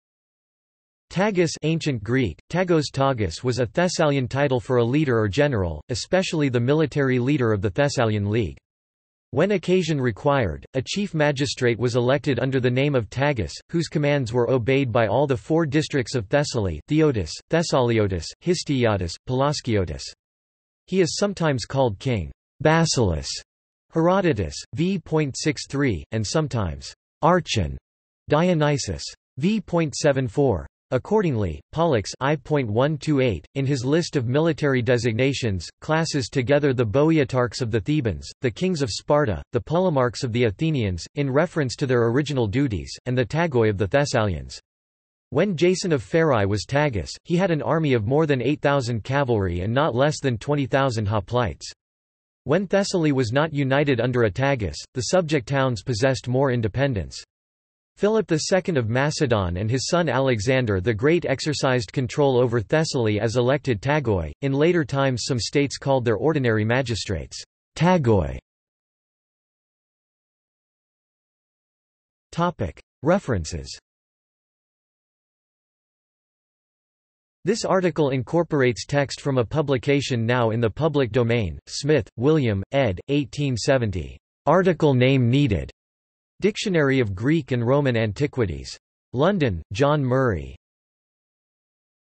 Tagus (title) | Wikipedia audio article
This is an audio version of the Wikipedia Article:
Tagus (title)

- Socrates

SUMMARY
Tagus (Ancient Greek: τᾱγός, τάγης) was a Thessalian title for a leader or general, especially the military leader of the Thessalian League. When occasion required, a chief magistrate was elected under the name of Tagus, whose commands were obeyed by all the four districts of Thessaly (Phthiotis, Thessaliotis, Histiaeotis, Pelasgiotis). He is sometimes called king ("basileus", Herodotus, V.63), and sometimes "archon" (Dionysius. V.74). Accordingly, Pollux (I.128), in his list of military designations, classes together the Boeotarchs of the Thebans, the Kings of Sparta, the Polemarchs of the Athenians, (in reference to their original duties), and the Tagoi of the Thessalians. When Jason of Pherae was Tagus, he had an army of more than 8000 cavalry and not less than 20,000 hoplites. When Thessaly was not united under a Tagus, the subject towns possessed more independence. Philip II of Macedon and his son Alexander the Great exercised control over Thessaly as elected Tagoi.  In later times some states called their ordinary magistrates "Tagoi".